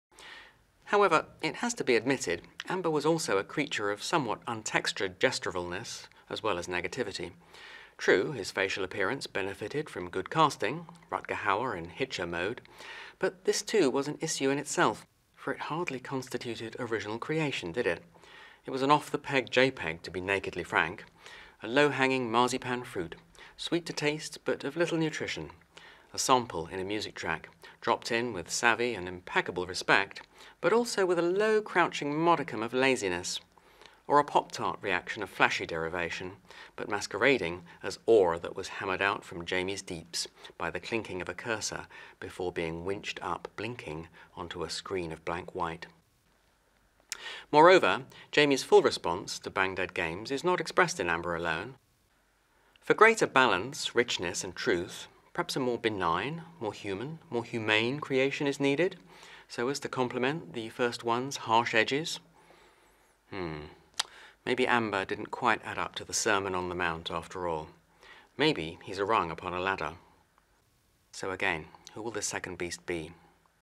Teaser 17(ii), marzipan fruit – for Rohan Quine’s “The Beasts of Electra Drive”
However, it has to be admitted Amber was also a creature of somewhat untextured gesturalness, as well as negativity. True, his facial appearance benefited from good casting—Rutger Hauer in Hitcher mode. But this, too, was an issue in itself: for it hardly constituted original creation, did it? It was an off-the-peg JPEG, to be nakedly frank. A low-hanging marzipan fruit, sweet to taste but of little nutrition; a sample in a music track, dropped in with savvy and impeccable respect but also with a low-crouching modicum of laziness; or a pop-tart reaction of flashy derivation, but masquerading as ore that was hammered out from Jaymi’s deeps by the clinking of a cursor before being winched up, blinking, onto a screen of blank white.

 Moreover, Jaymi’s full response to Bang Dead Games is not expressed in Amber alone. For greater balance, richness and truth, perhaps a more benign, more human, more humane creation is needed, so as to complement the first one’s harsh edges?

 Hmm. Maybe Amber didn’t quite add up to the sermon on the mount, after all. Maybe he’s a rung upon a ladder.

 So again, who will this second Beast be?